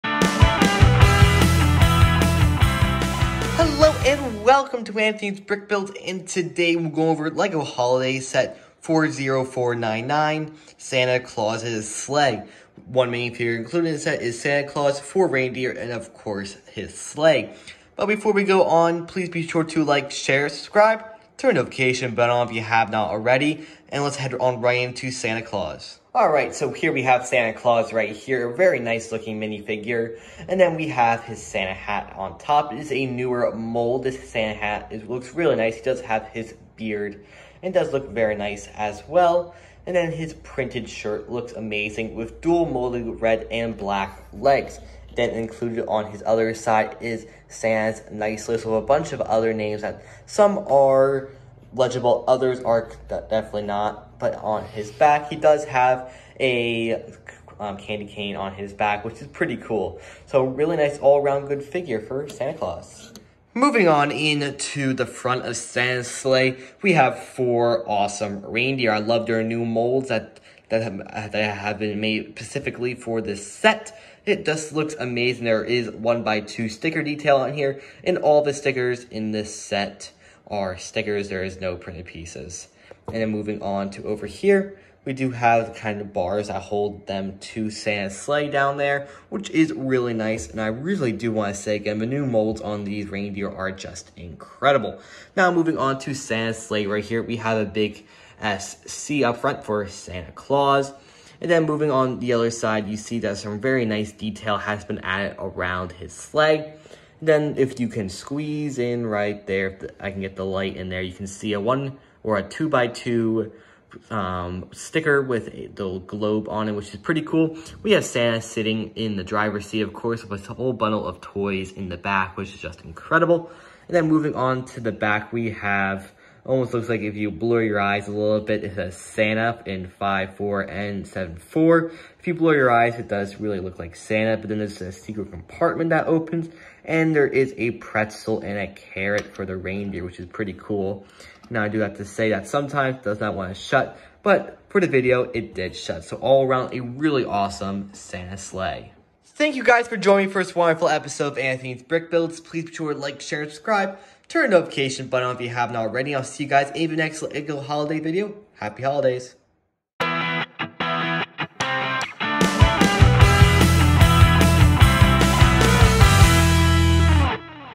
LEGO Santa's Sleigh (40499) Review!
Today we will be going over the Santa's Sleigh set! This is my fifth and last of my five Christmas videos.